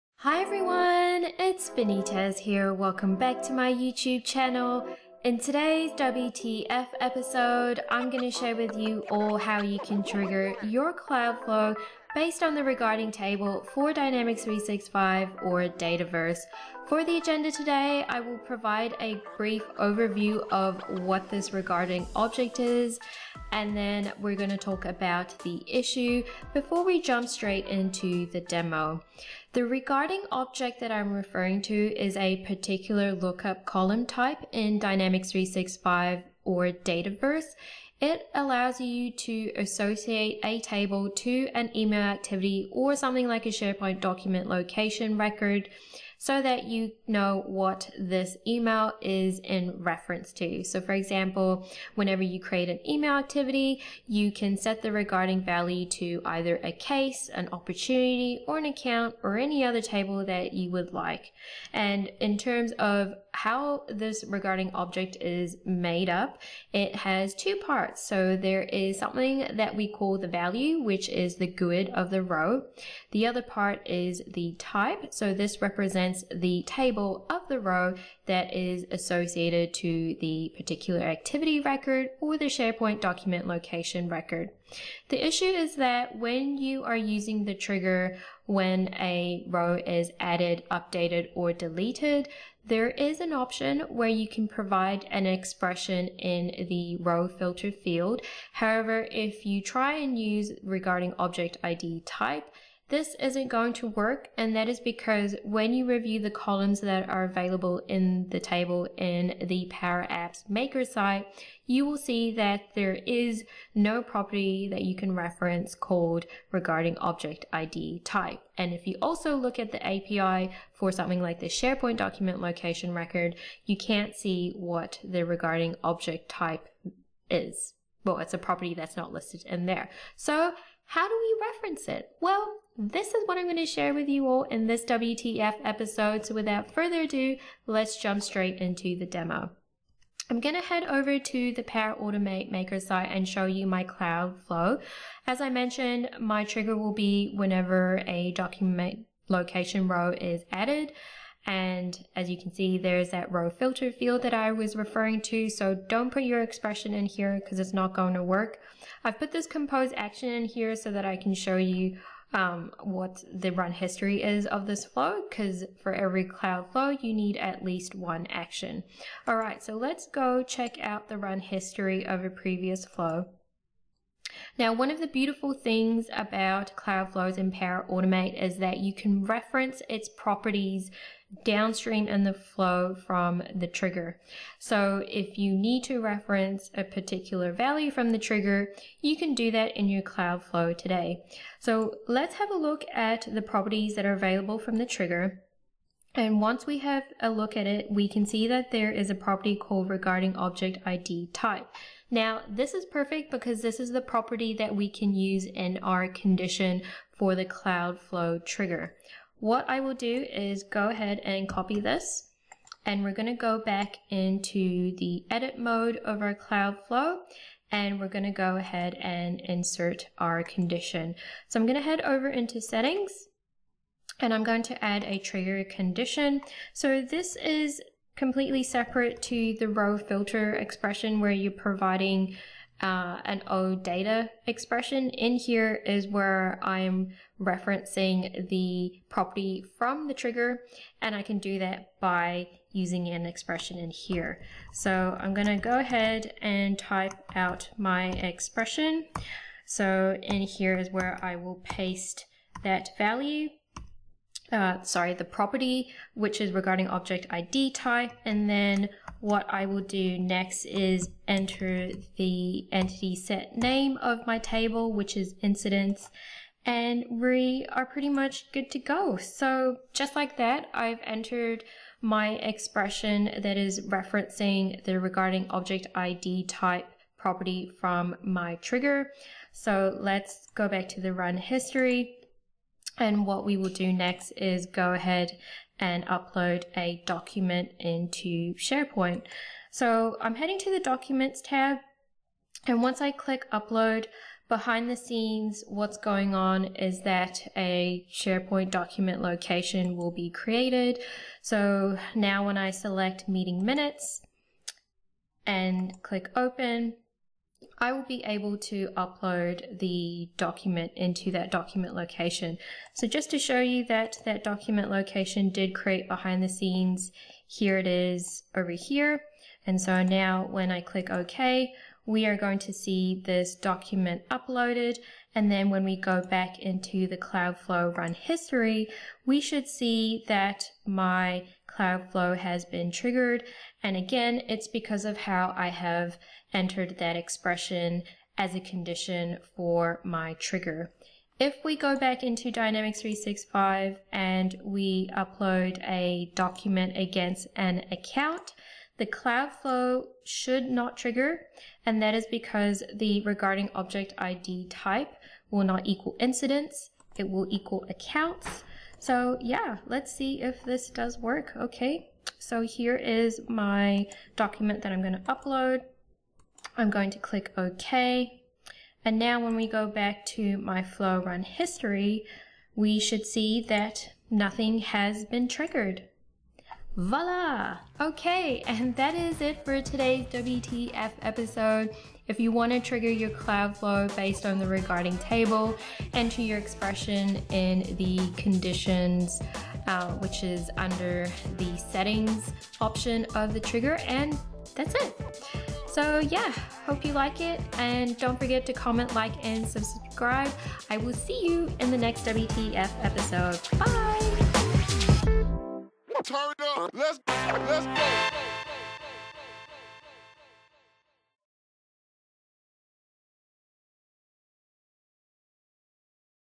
Trigger cloud flows based on the Regarding object type for Dynamics 365 or Dataverse
In Dynamics 365 or Dataverse you'll notice a lookup column called "Regarding" which allows you to associate another row from another table. For example, a document uploaded into SharePoint from Dynamics 365 is in regards to a Case. In this WTF episode I'll show you how to trigger a cloud flow for Dynamics 365 or Dataverse based on the table in the lookup column which is also know as the Regarding object table.

00:00​ - Introduction
00:16​ - Agenda
02:20​ - What the cloud flow looks like
02:49​ - Run History to review the trigger properties
03:29​ - Using the property in an expresison for the Trigger Condition
04:26​ - Cloud flow in action